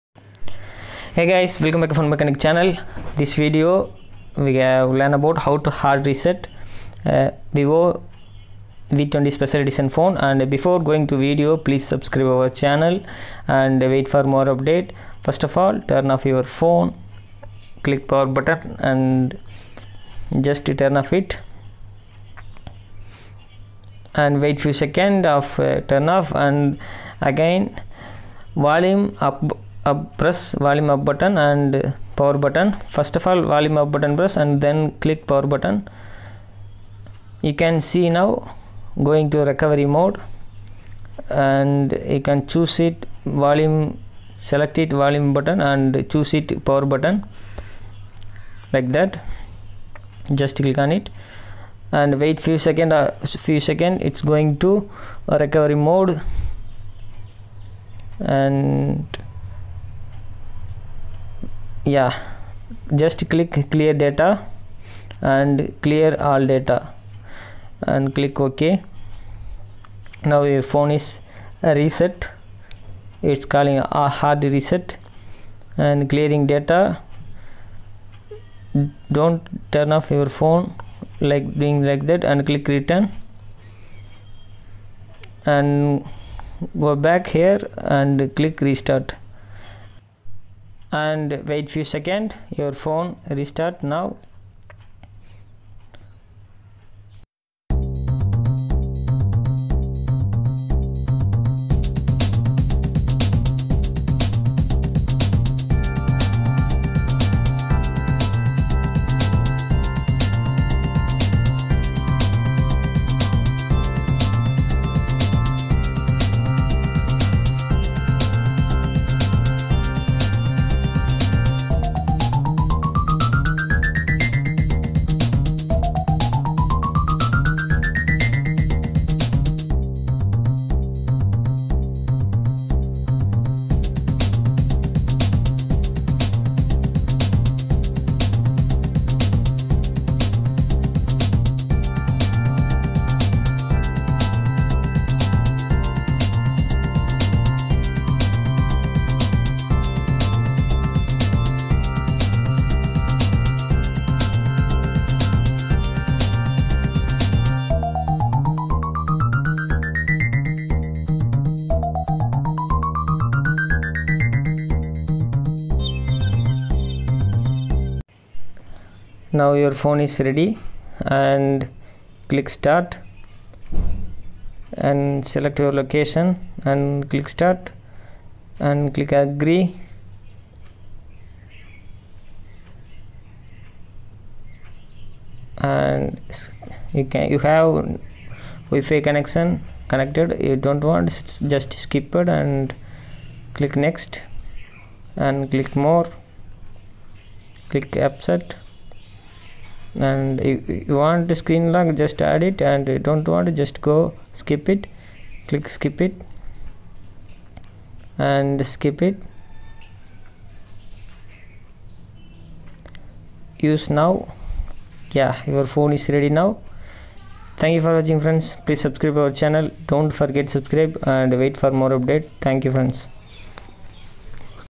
How to Hard Reset In VIVO V20 SE mobile|Master Format| factory reset|VIVO V2
In today's video i am going to show How to Hard Reset In VIVO V20 SE mobile phone full guide watch full video for acqurecy. unless you have to face a problem..